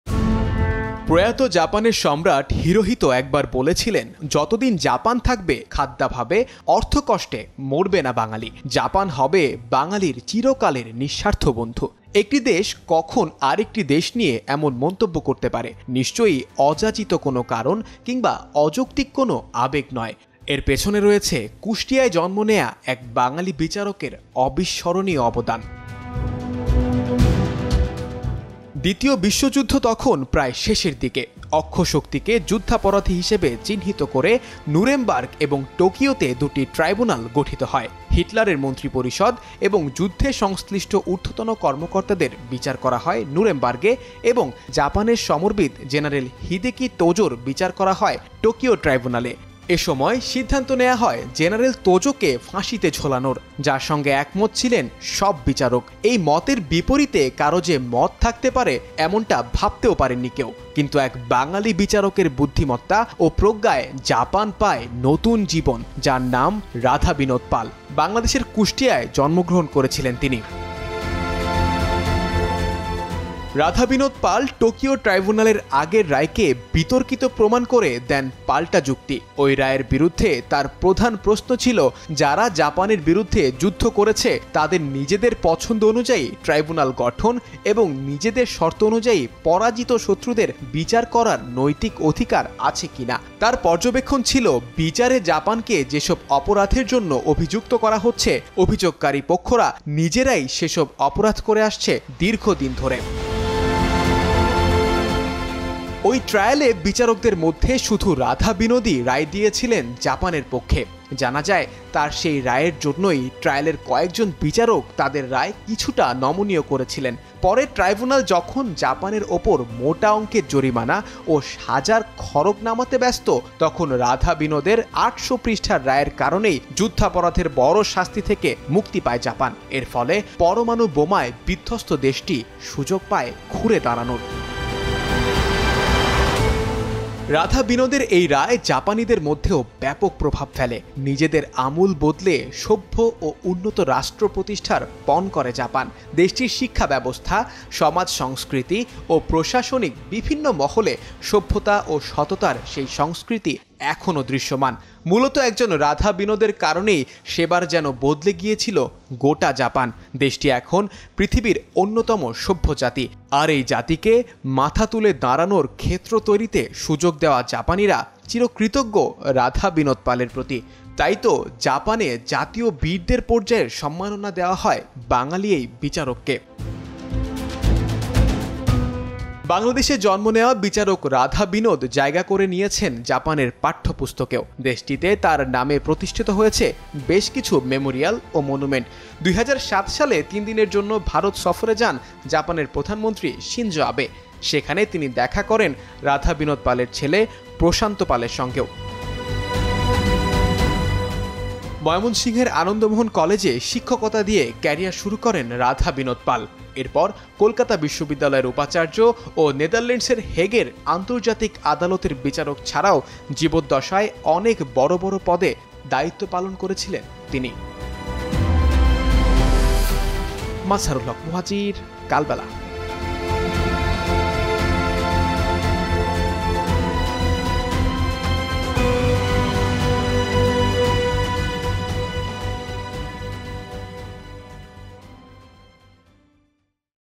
যে বিচারকের জন্য বাংলাদেশের নিঃস্বার্থ বন্ধু জাপান | Japan Bangladesh Friendship | Kalbela News
প্রয়াত জাপানের সম্রাট হিরোহিতো একবার বলেছিলেন, "যতদিন জাপান থাকবে, খাদ্যাভাবে, অর্থকষ্টে মরবে না বাঙালি। জাপান হবে বাঙালির চিরকালের নিঃস্বার্থ বন্ধু।" একটি দেশ কখন আরেকটি দেশ নিয়ে এমন মন্তব্য করতে পারে? নিশ্চয়ই অযাচিত কোনো কারণ বা অযৌক্তিক কোনো আবেগ নয়, এর পেছনে রয়েছে কুষ্টিয়ায় জন্ম নেওয়া এক বাঙালী বিচারকের অবিস্মরণীয় অবদান।